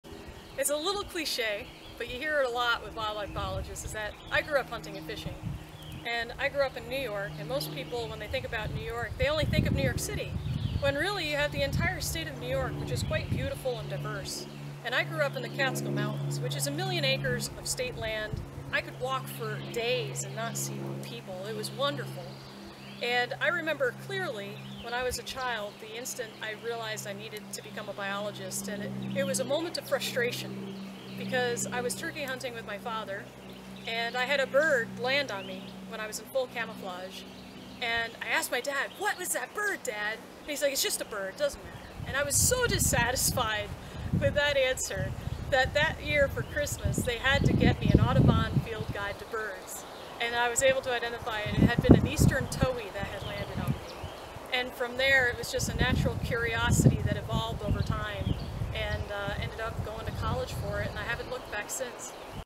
ABAC's Dr. Vanessa Lane
Associate professor Dr. Vanessa Lane of Abraham Baldwin Agricultural College tells how she became interested in the outdoors and wildlife biology as a child growing up in New York. (Credit: Josiah Lavender/Georgia DNR. Credit required for use.)

This video was featured in the Georgia Wild E-newsletter.

Follow Georgia DNR's Wildlife Resources Division!